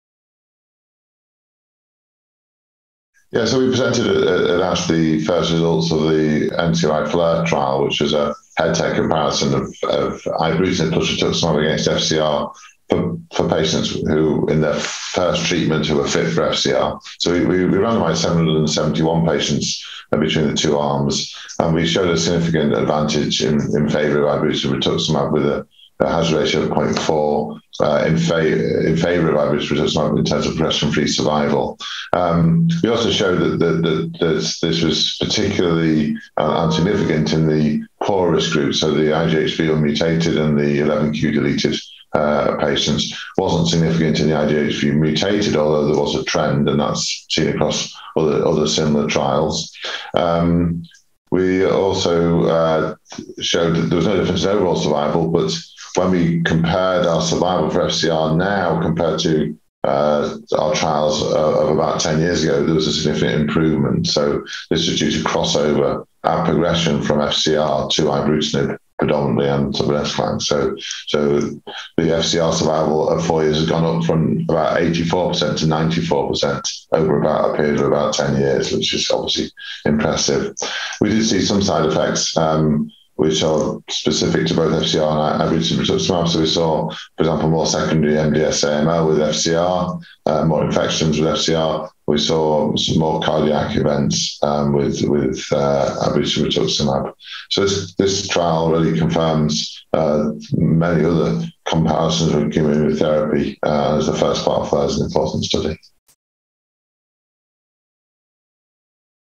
Updates from the FLAIR trial: IR vs. FCR for CLL
In this video, Peter Hillmen, MBChB, FRCP, FRCPath, PhD, University of Leeds, Leeds, UK, presents a summary of the first results from the ongoing Phase III FLAIR trial (ISRCTN01844152), which aims to compare the use of ibrutinib and rituximab (IR) versus fludarabine, cyclophosphamide and rituximab (FCR) in the treatment of patients with chronic lymphocytic leukemia (CLL). Dr Hillmen explains the significant advantages observed when using IR, including improved progression-free survival (PFS), especially in the IGHV unmutated and (del)11q subset of patients. To conclude, Dr Hillmen discusses some side effects that were seen in both treatment regimens, such as cardiac events and secondary myelodysplastic syndromes/ acute myeloid leukemia (MDS/AML). This interview took place at the 63rd ASH Annual Meeting and Exposition Congress, Atlanta, GA, 2021.